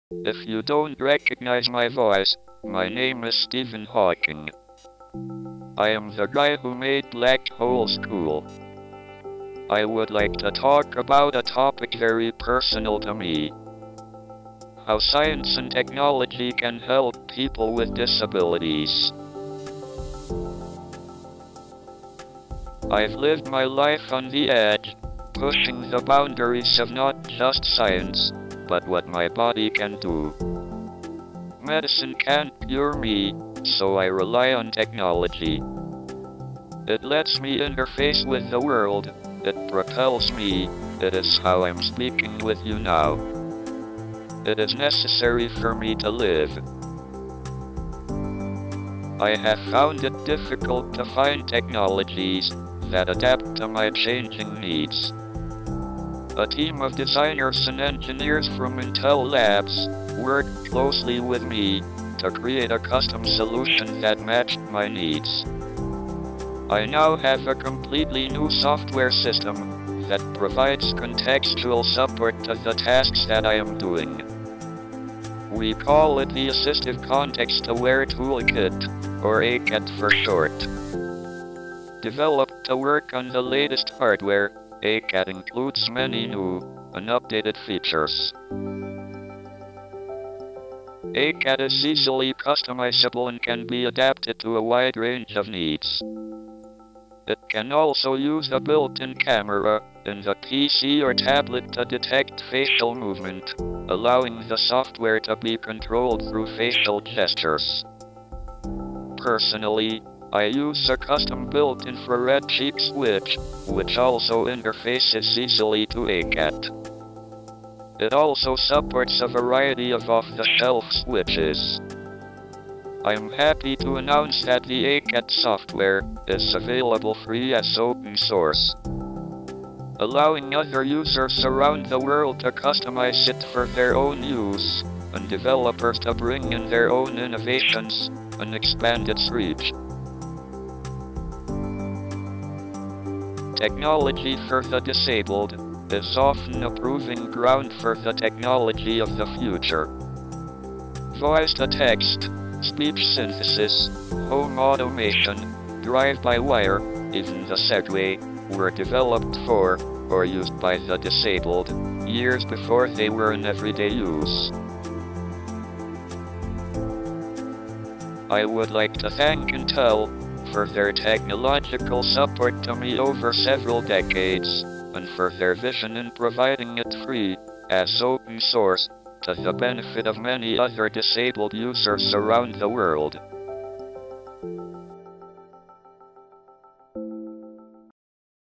Stephen Hawking talks about technology and ACAT (Assistive Context Aware Tool-kit)
Stephen Hawking explains how the disabled community can often influence future technologies.